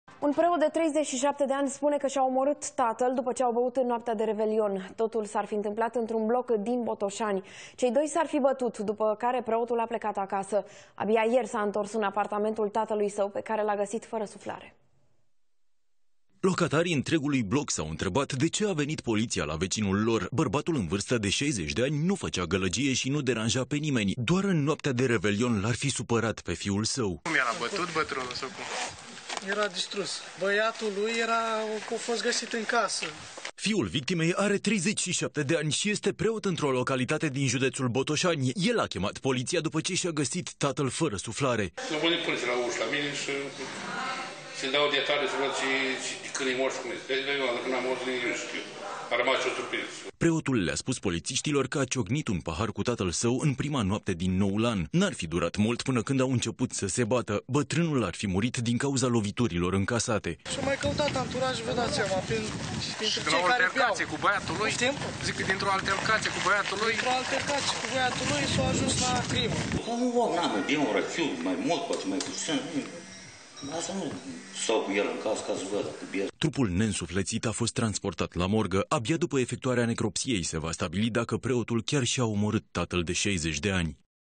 Preot suspectat că şi-a ucis tatăl
Un preot de 37 de ani spune că şi-a omorât tatăl, după ce au băut în noaptea de revelion. Totul s-ar fi întâmplat într-un bloc din Botoşani. Cei doi s-ar fi bătut, după care preotul a plecat acasă. Abia ieri s-a întors în apartamentul tatălui său, pe care l-a găsit fără suflare., Stiri, Antena 1, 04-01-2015